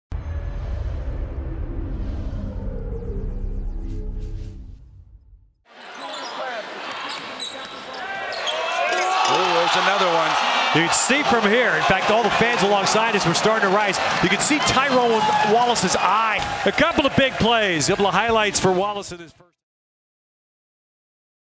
Highlight: Cal's Tyrone Wallace gets a monster dunk
Cal men's basketball guard Tyrone Wallace gets a monster slam in the Bears' exhibition opener against Carroll.